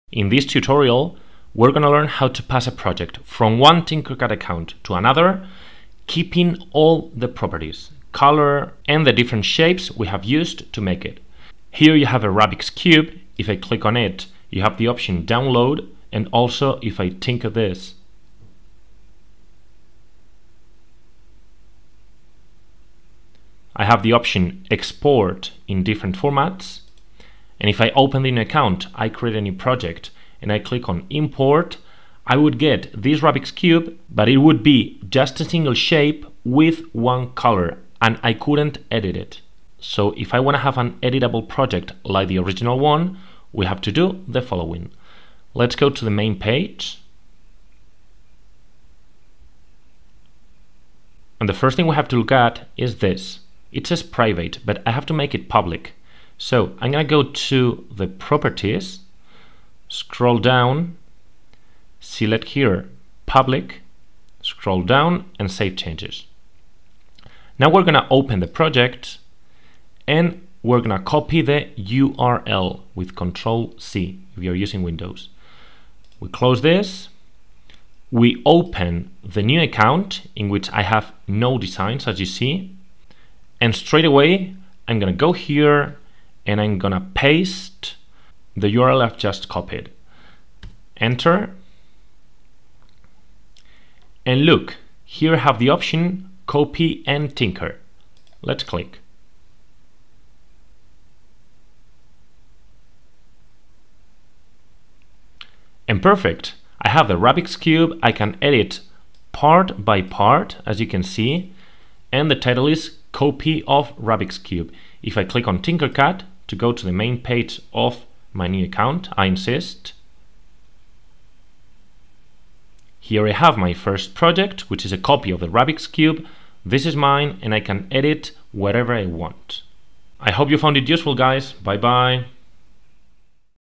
How to TRANSFER TINKERCAD DESIGNS between accounts MAINTAINING THE PROPERTIES | Design → Account
In this video, we explain how to move a Tinkercad project from one account to another making sure it keeps all its properties and is fully editable. In addition, all this without the need to download anything.